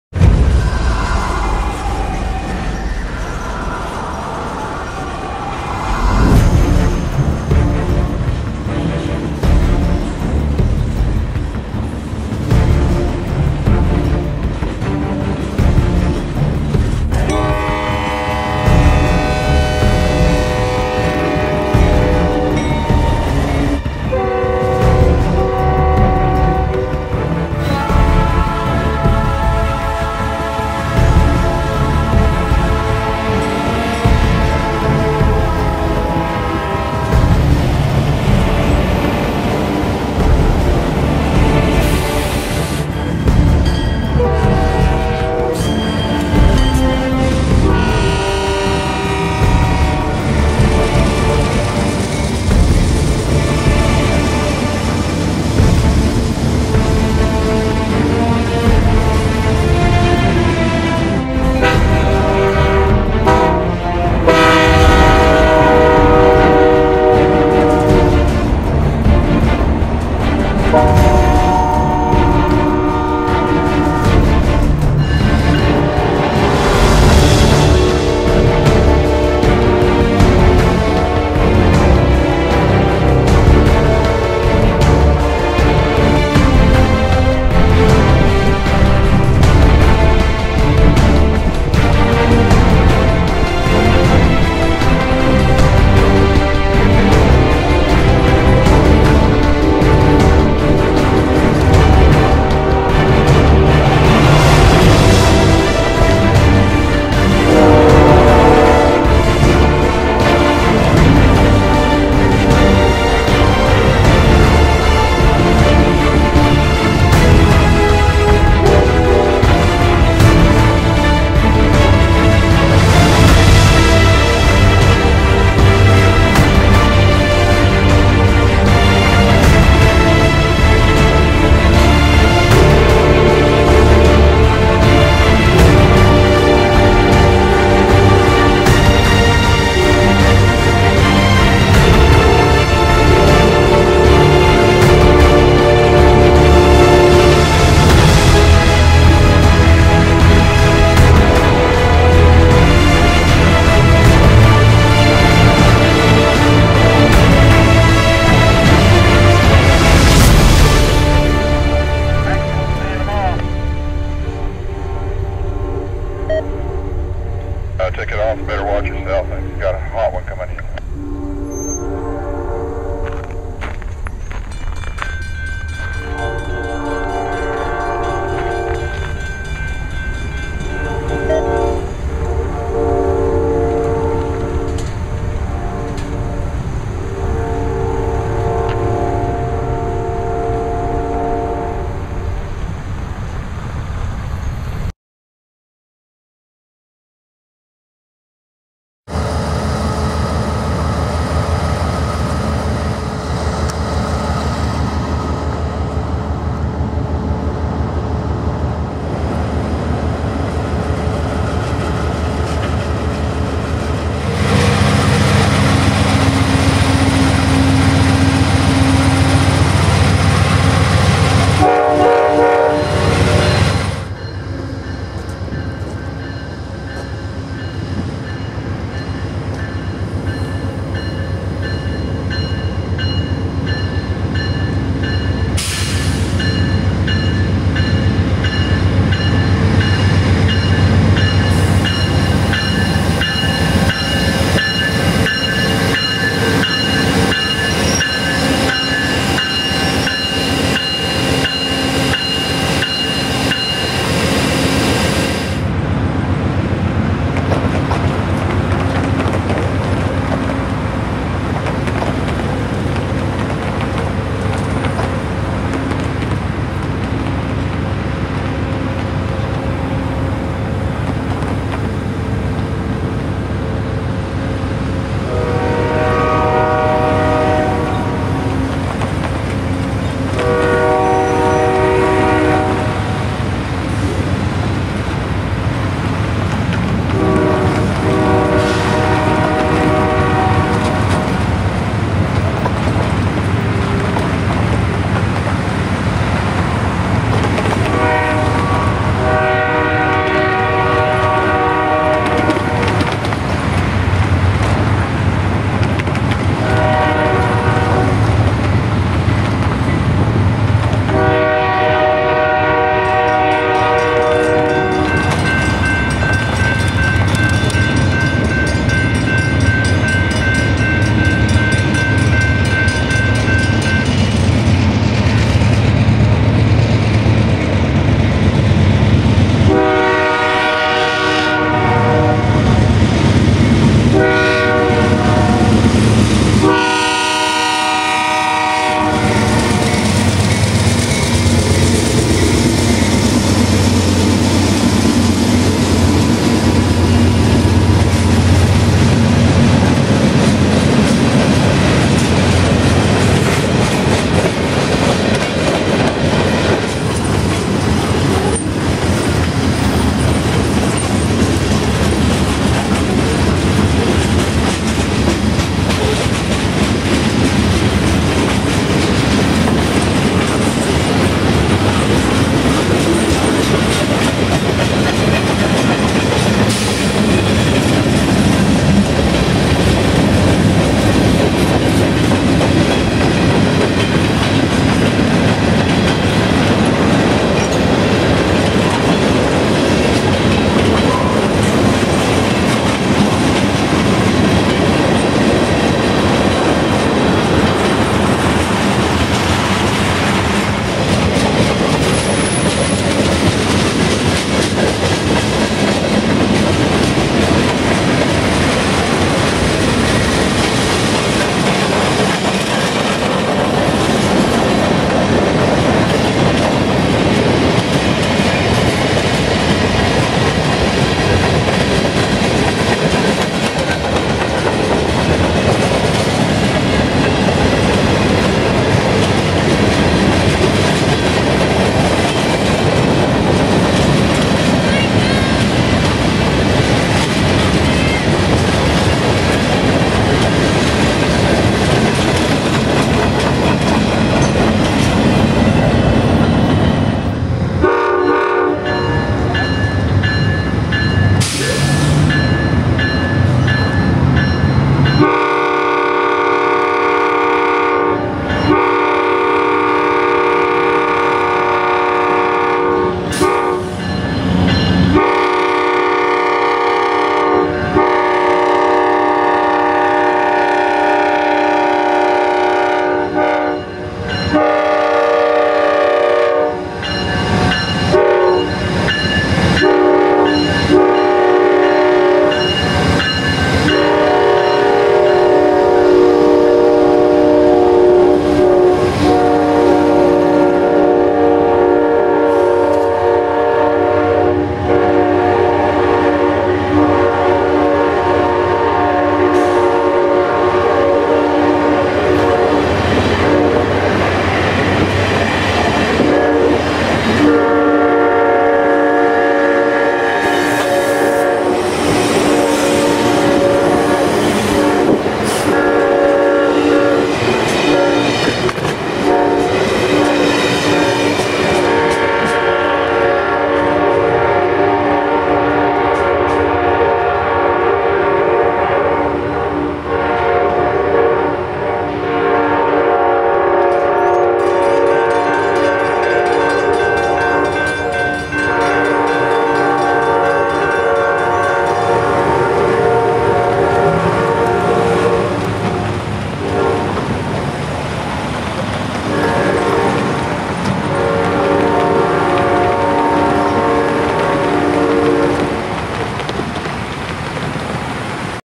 Railfanning in Carbondale, IL (1-25-11)
Train 1: AMTK P39291-25
AMTK GE P42DC #31

Train 2: CN M33681-25
CN GE C44-9W #2672
CN GE C44-9WL #2517